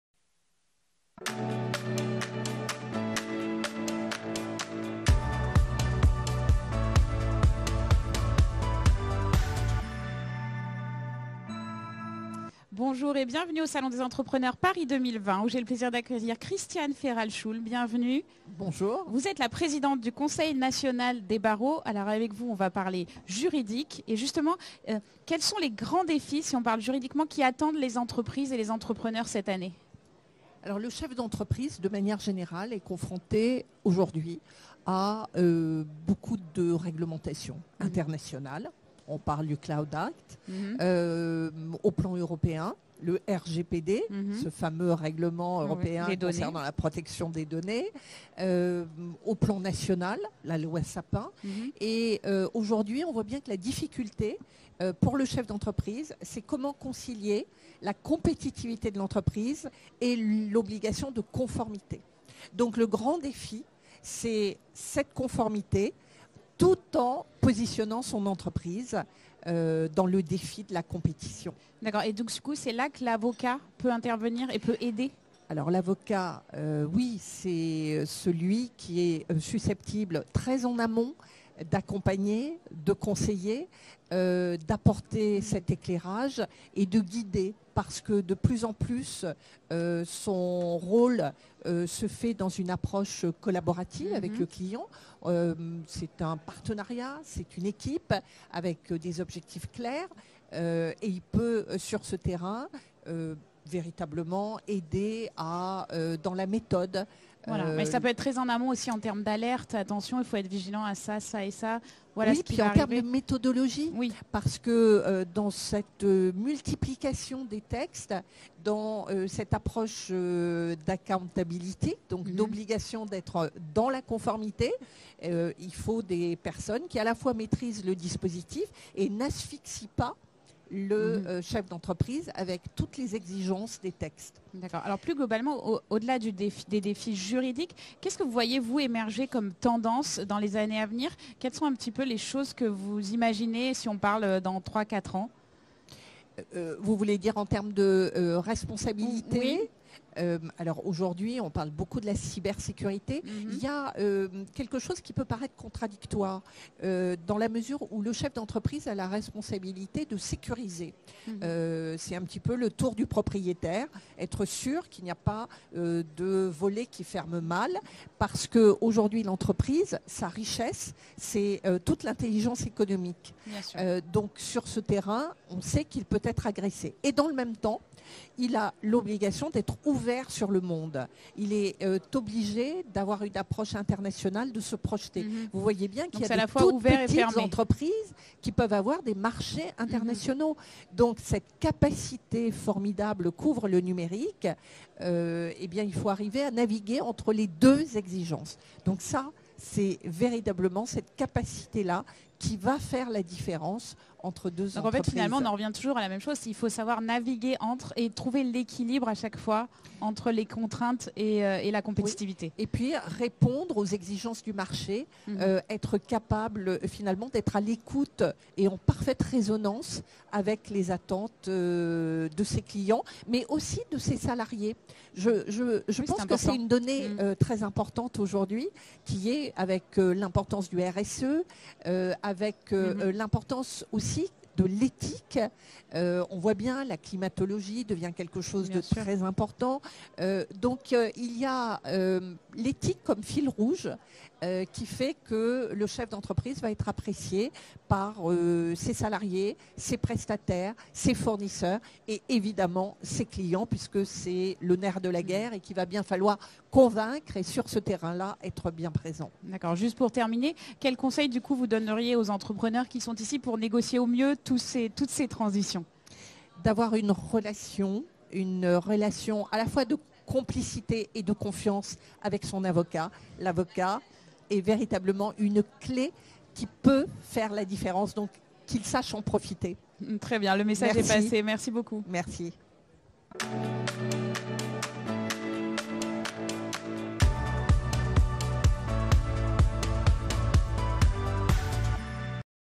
Interview de Christiane Féral-Schuhl au Salon des Entrepreneurs 2020
A l'occasion du Salon des Entrepreneurs 2020, Christiane Féral-Schuhl, présidente du CNB, a évoqué les défis qui attendent les entreprises et les entrepreneurs d'un point de vue juridique.